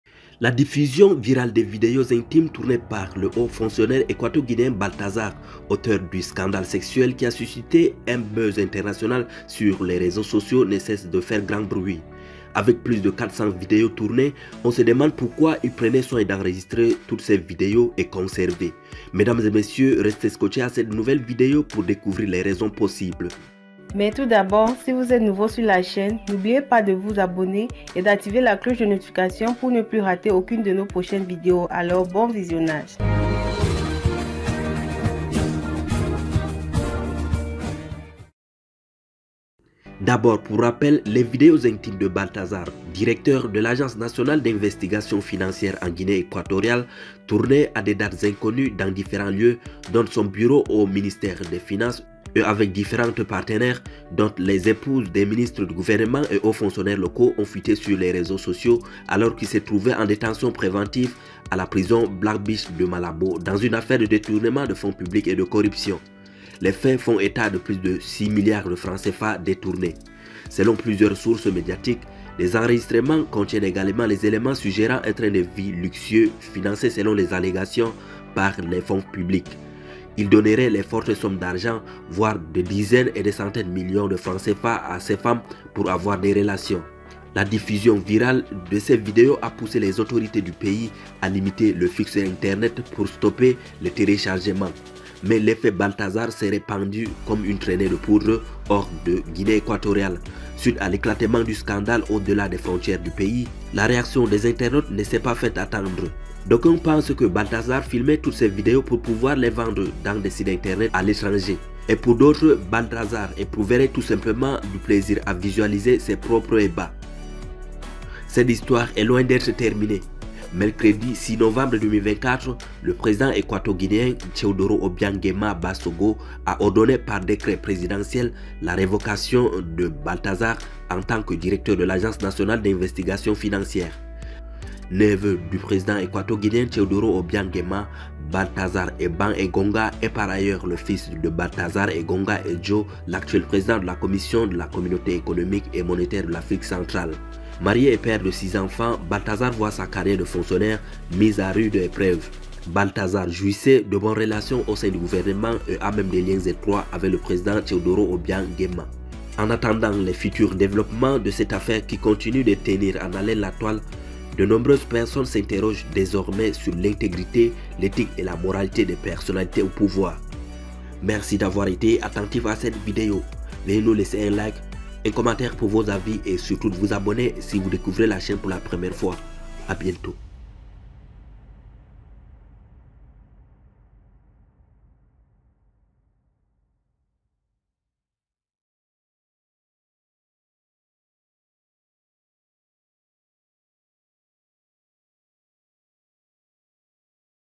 Voici pourquoi Balthazar enregistrait les 400 vidéos I magazine du savoir
Baltasar, un haut fonctionnaire en Guinée équatoriale. Découvrez son histoire dans cette nouvelle vidéo